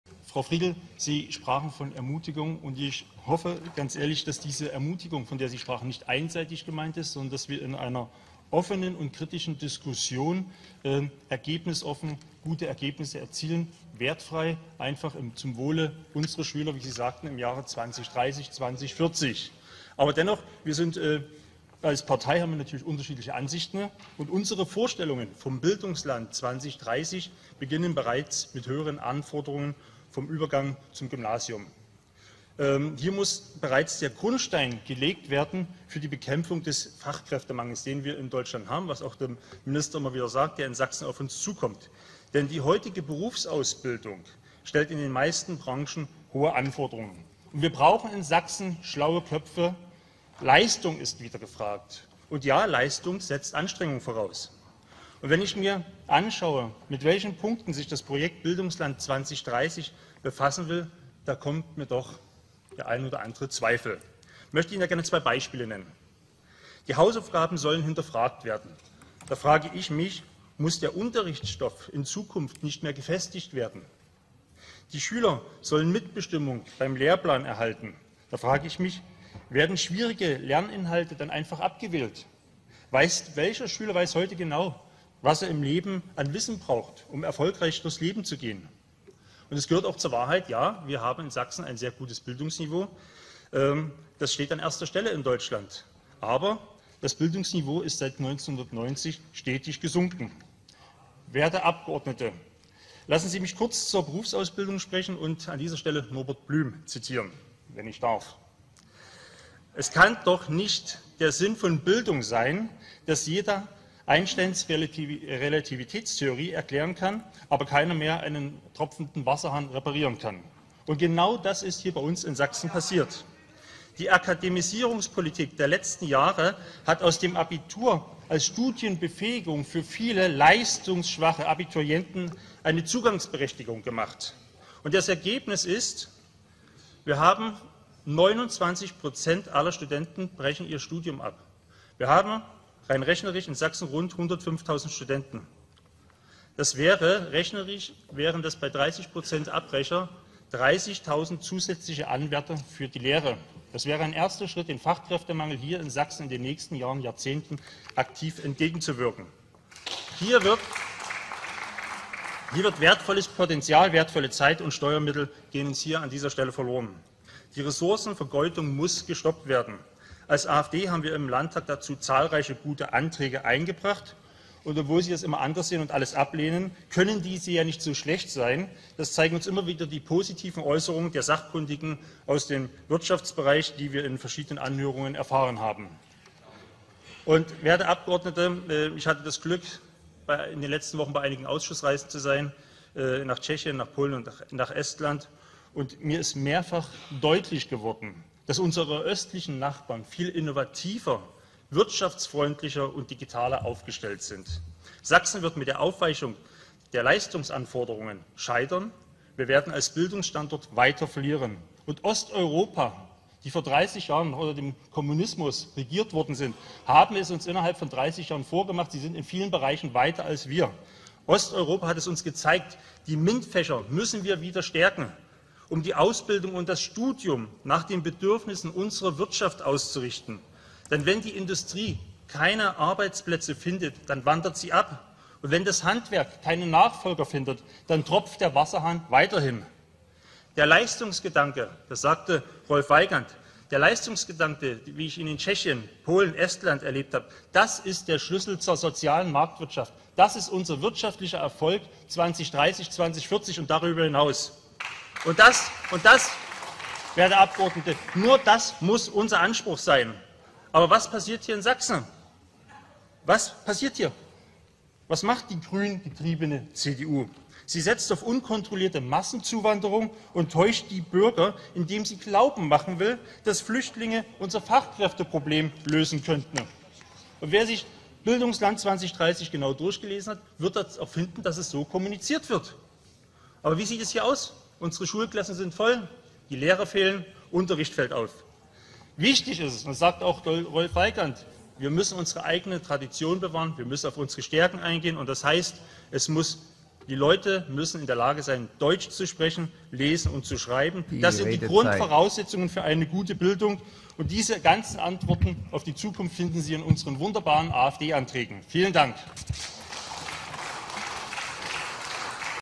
Der Leistungsgedanke ist der Schlüssel zum Erfolg der sozialen Marktwirtschaft
Aktuelle Debatte am 1.6.2023 zum Projekt "Bildungsland Sachsen 2030" der Staatsregierung in Sachsen. Während CDU-SPD-Grüne soziale Aspekte in der Bildung favorisieren, setzen wir klar auf Leistungsbereitschaft. Die Akademisierungspolitik der letzten Jahre hat aus dem Abitur als Studienbefähigung für viele leistungsschwache Abiturienten eine Zugangsberechtigung gemacht.
Das Ergebnis:
- Circa 29 % brechen ihr Studium ab!
Bei rund 105.000 Studenten in Sachsen wären dies rein rechnerisch ca. zusätzliche 30.000 Anwärter für eine Berufsausbildung.